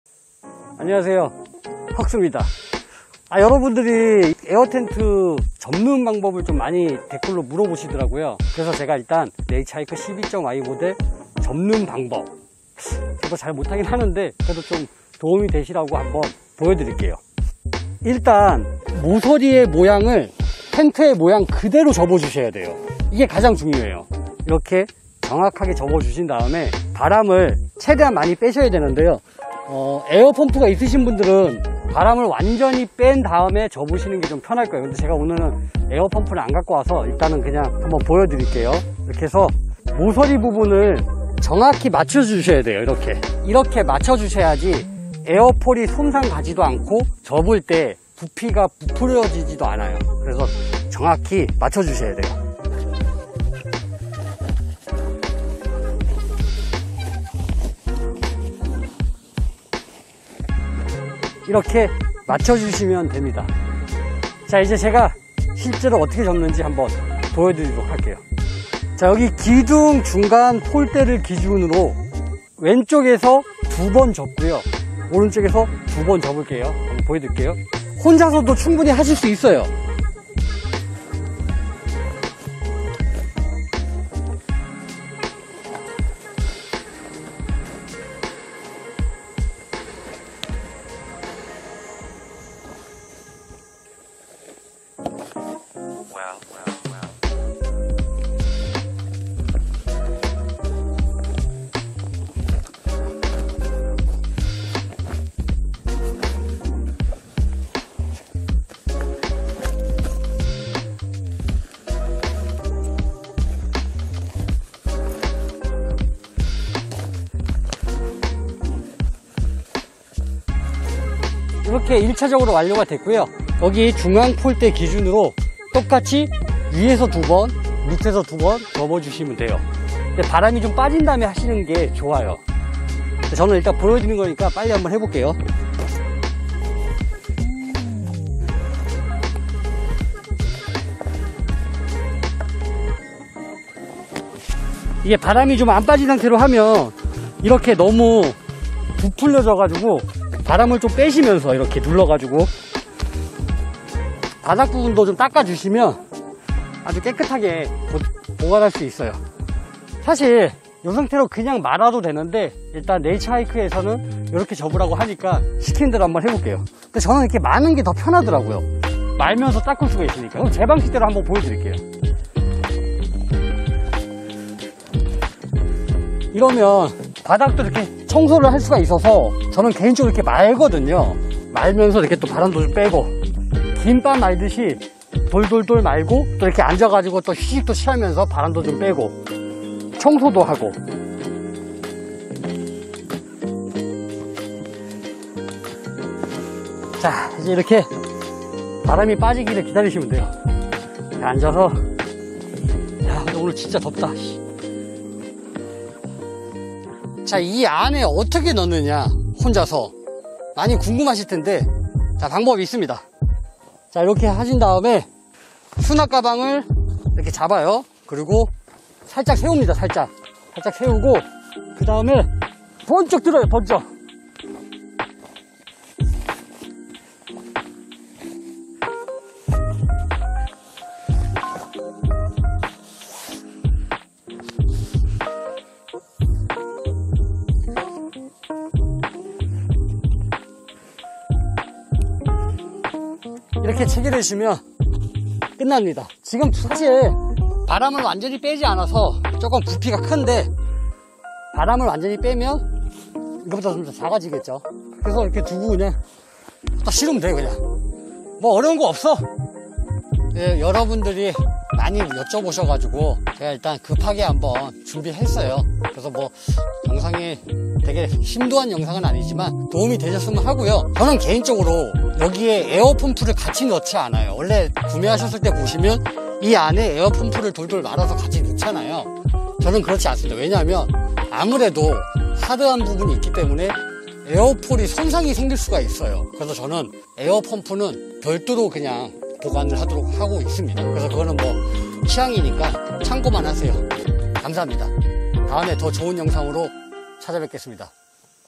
대형 에어텐트 혼자 접는 방법 | 네이처하이크 12y 에어텐트 접는법
🎪 4K 고화질 영상으로 시청하세요!!
안녕하세요. 에어텐트를 사용하는 많은 캠퍼님들이 접는법을 잘 몰라서 고생하신다는 댓글이 많아 준비했습니다.
네이처하이크 포함 모든 에어텐트 접는 방법은 비슷합니다.
영상을 보시고 도움이 되시길 바랍니다.
에어텐트 접는법에 사용된 텐트는 네이처하이크 에어텐트 12Y 모델입니다.